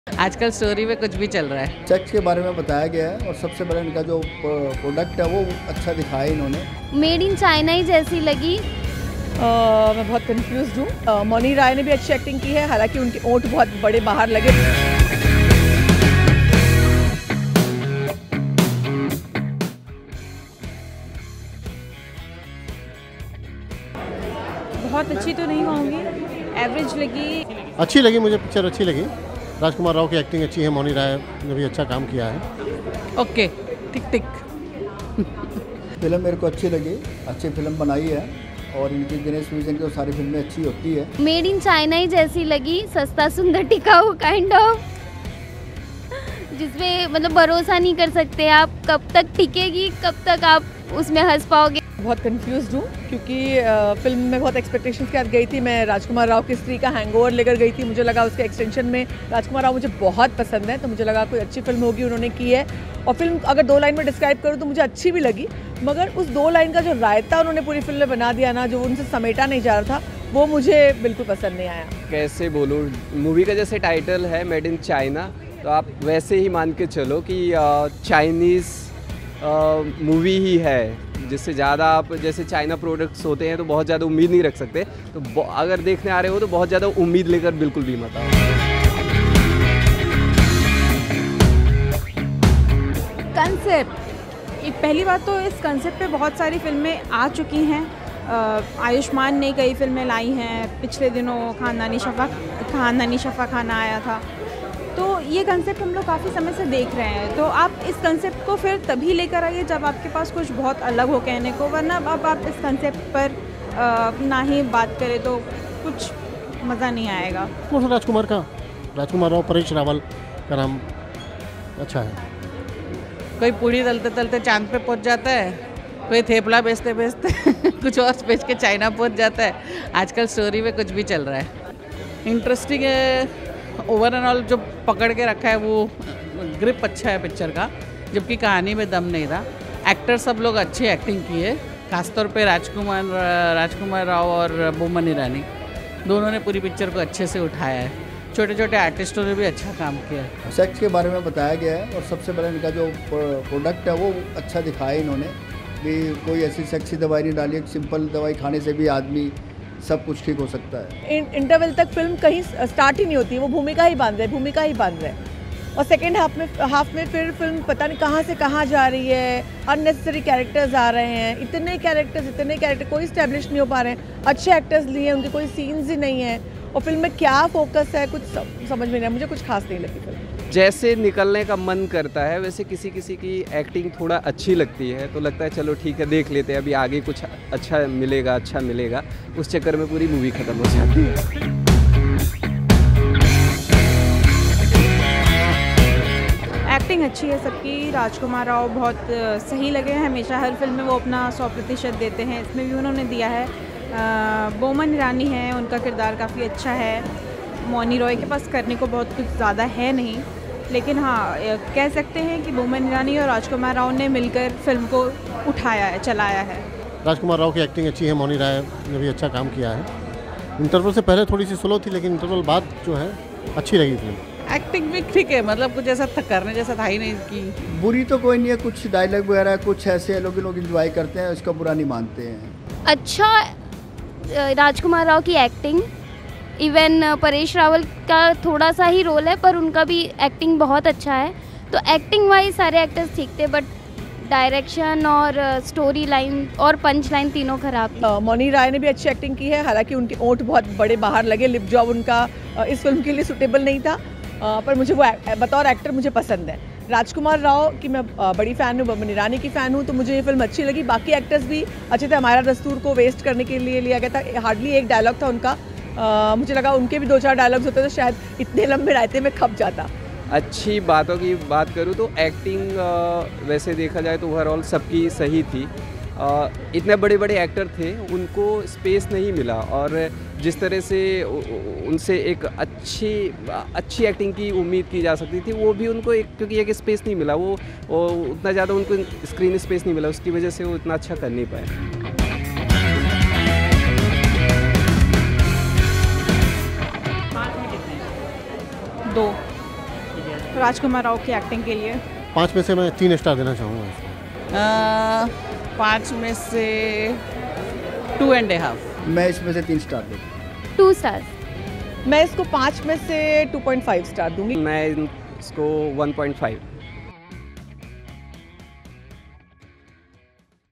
Public Review: Made in China in Hindi | Rajkummar Rao Mouni Roy Boman Irani Paresh Rawal Gajraj Rao
Public review of Made in China in Hindi starring Rajkummar Rao, Mouni Roy, Boman Irani, Paresh Rawal and Gajraj Rao.